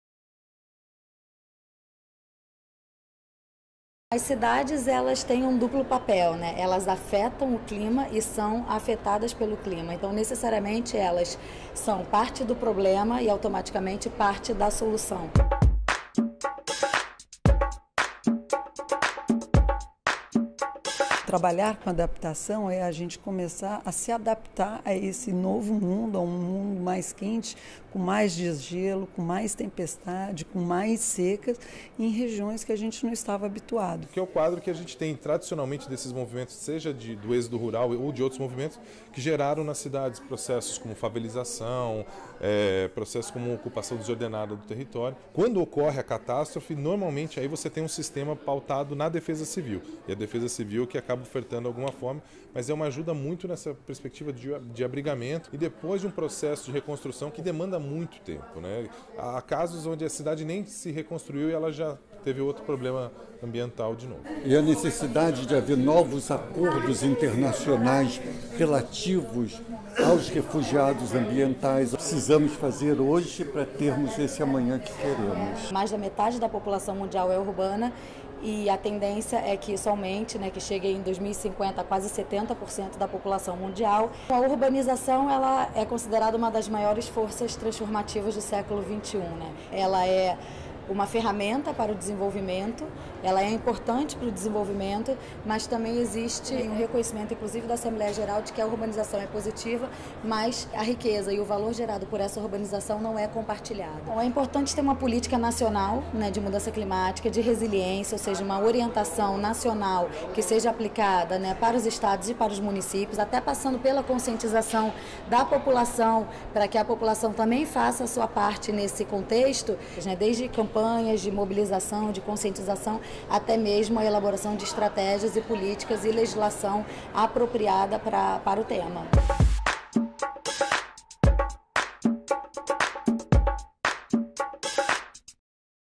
Diálogos estratégicos em mudanças do clima: como as cidades se preparam?
O impacto das mudanças climáticas nos movimentos migratórios globais e a capacidade de resposta das cidades foram temas de debates do “Diálogo Vidas Deslocadas – Mudanças do Clima e Mobilidade Humana”, que aconteceu no Museu do Amanhã em 2017 e uniu atores-chave para debater mudanças climáticas e mobilidade humana, criando um espaço de aprendizagem catalisador de ações.

O evento foi uma parceria entre a ONU Meio Ambiente, ACNUR (Agência da ONU para Refugiados), OIM (Agência das Nações Unidas para as Migrações), ONU-Habitat (Programa das Nações Unidas para os Assentamentos Humanos) e Museu do Amanhã. Detalhes